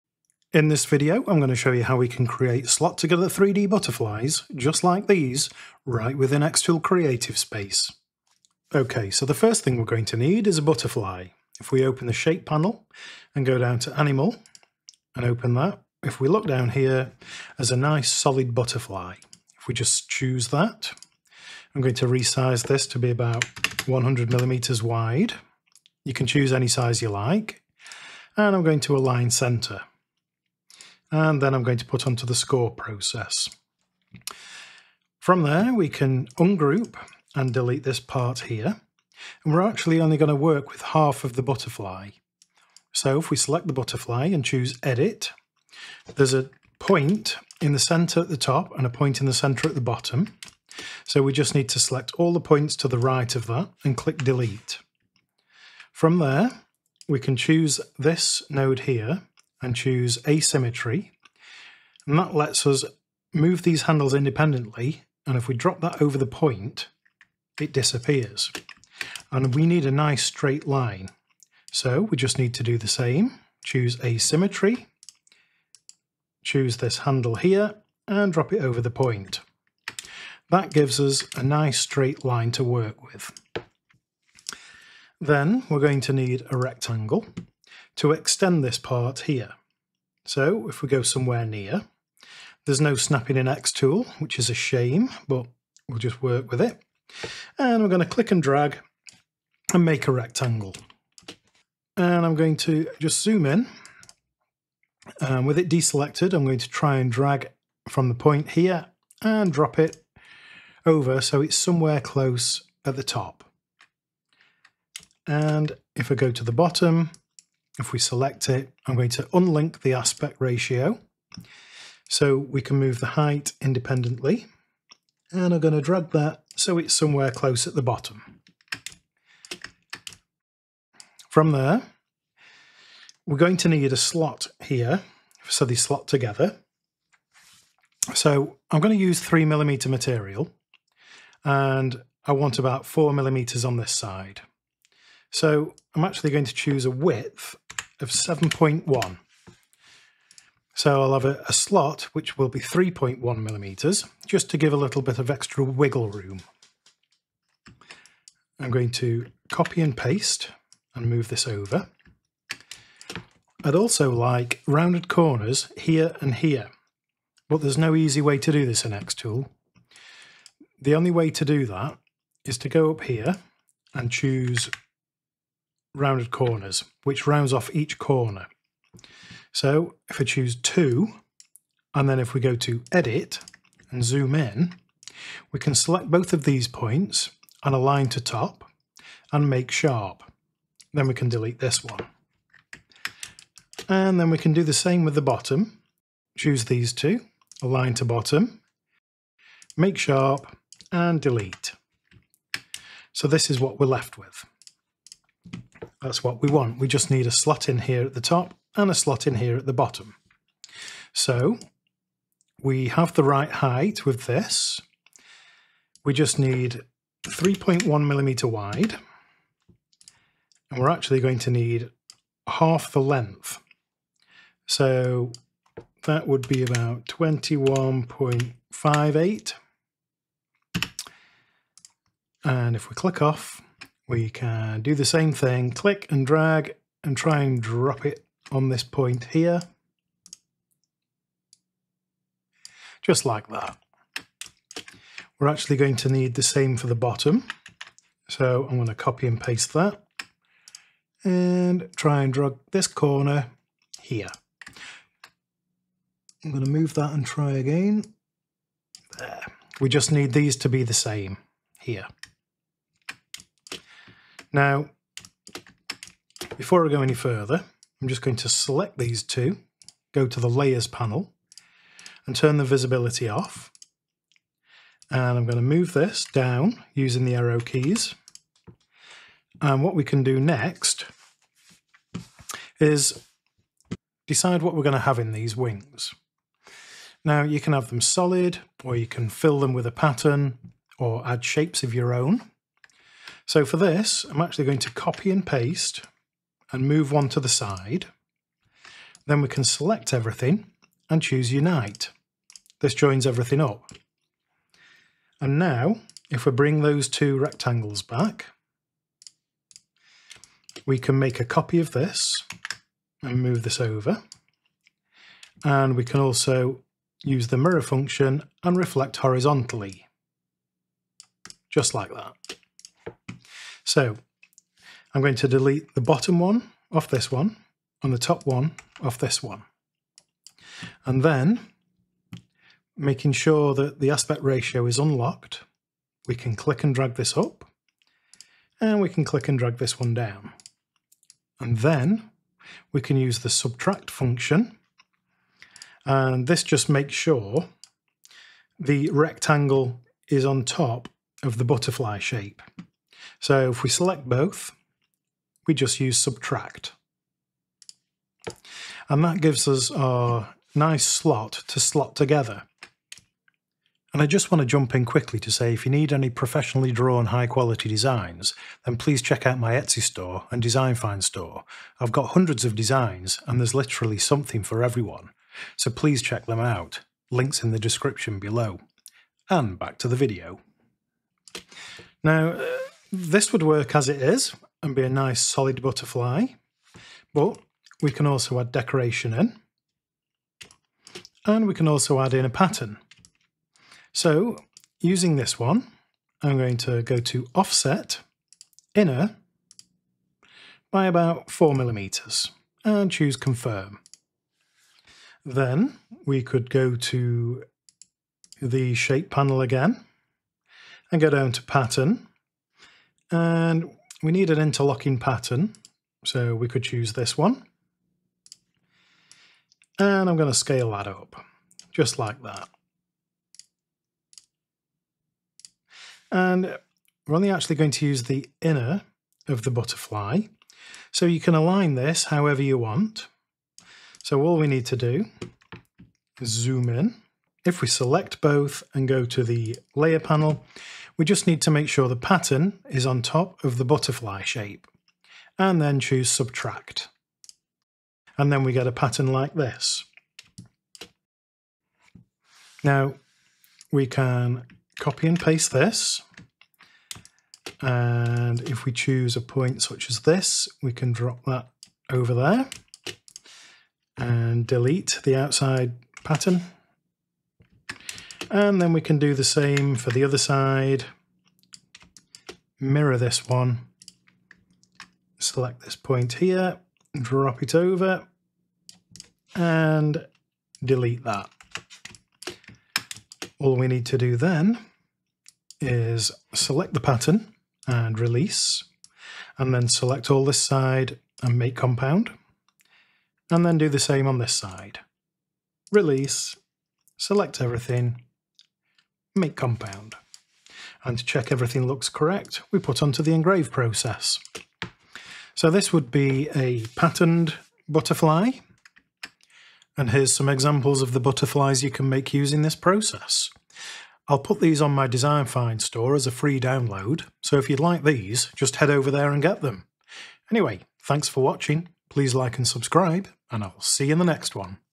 Make Beautiful 3D Butterflies in XTool Creative Space!
Learn how to create beautiful 3D Butterflies in XCS, with this XTool Creative Space 2.0 tutorial!
In this tutorial, we'll show you how to unleash your creativity to make custom slot together 3D Butterflies with ease.

From simple to complex patterns! Whether you're a graphic designer, crafter, or DIY enthusiast, this video is perfect for anyone looking to take their designs to the next level. So, let's dive in and explore the endless possibilities of slot together designs in XTool Creative Space!
Thanks Gav 😀

Timestamps:
0:00 - Start
3:25 - Adding The Slots
6:21 - Adding A Pattern